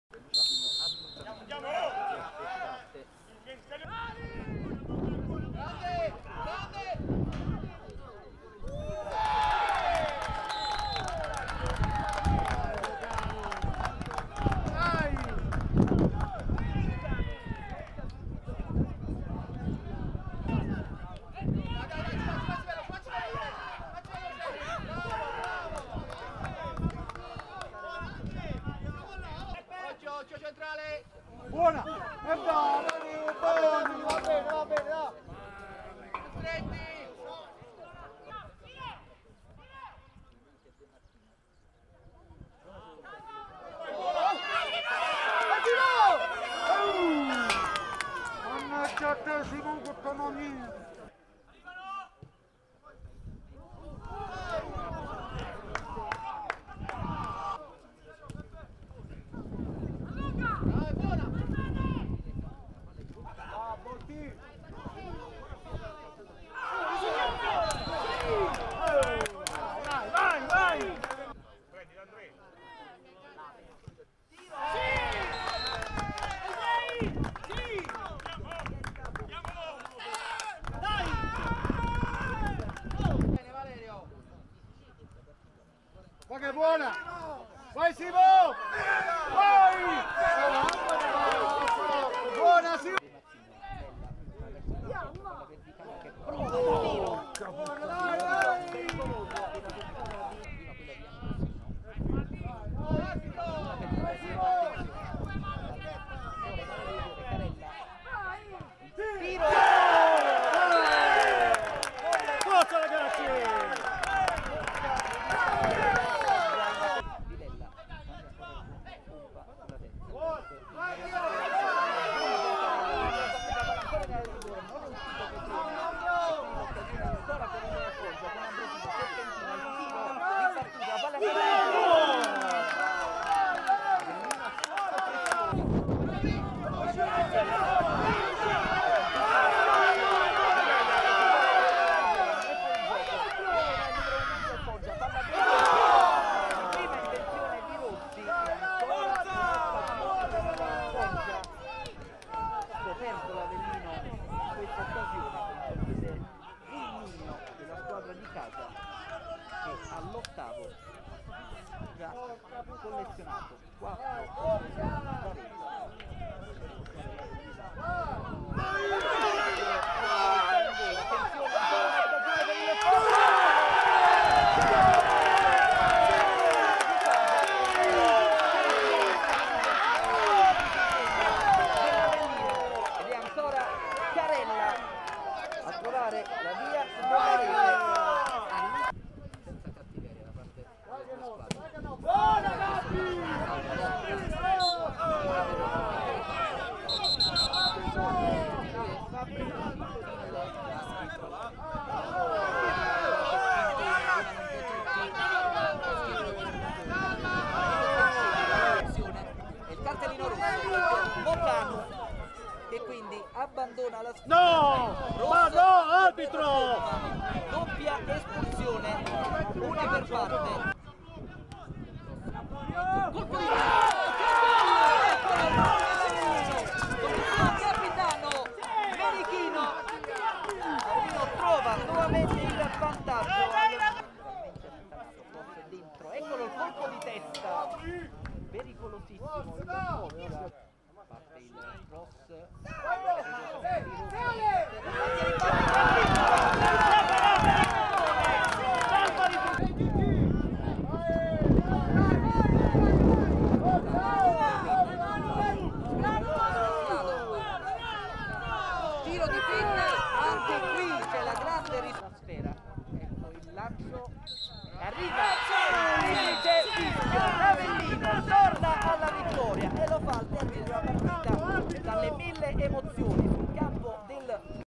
Highlights Foggia-Avellino 2-3: Gol e Azioni Salienti (Campionato Primavera 3)
Gli highlights e i gol realizzati nel corso dei 90 minuti di Foggia-Avellino 2-3 (Campionato Primavera 3).